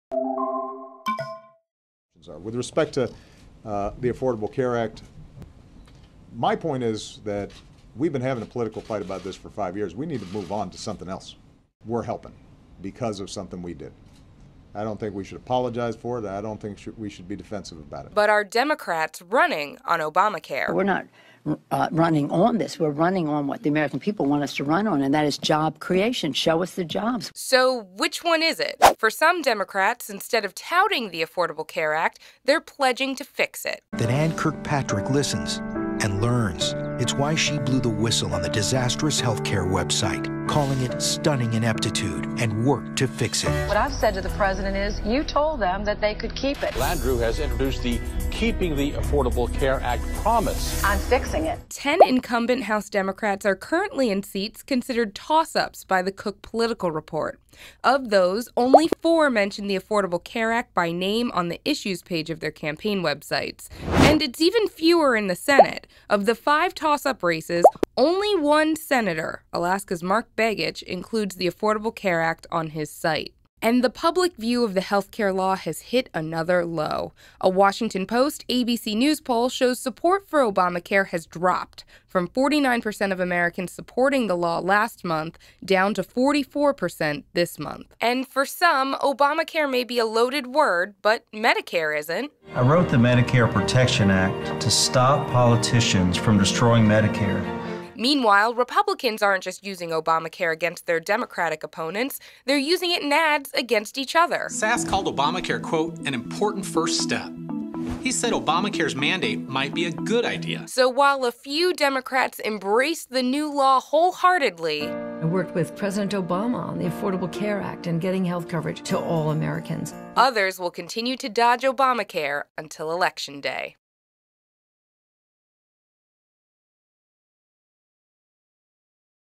Democrats' complex relationship with Obamacare
Some Democrats are using healthcare as a talking point for the midterms, while others are running from Obamacare. But can Dems run on the ACA without saying ACA at all?